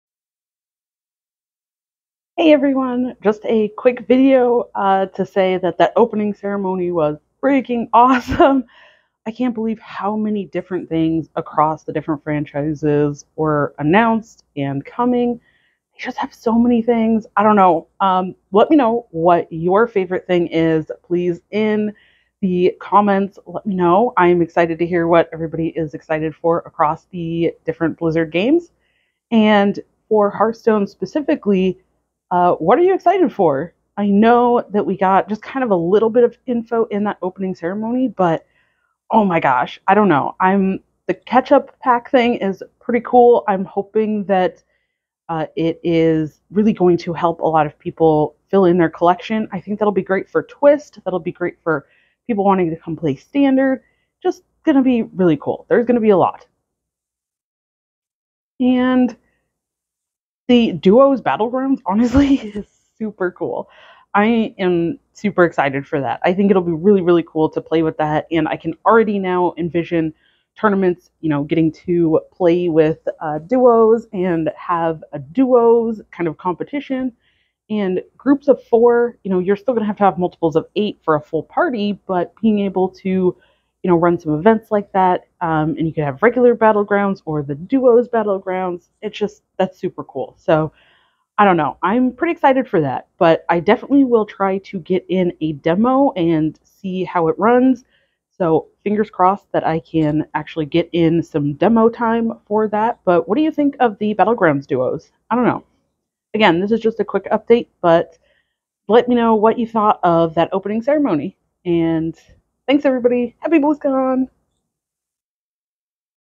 Quick Post-BlizzCon Opening Ceremony Thoughts!
BlizzCon opening ceremony quick thoughts between the BlizzCon action!

I'm excited to try some Battlegrounds Duos, what did you think of the announcements?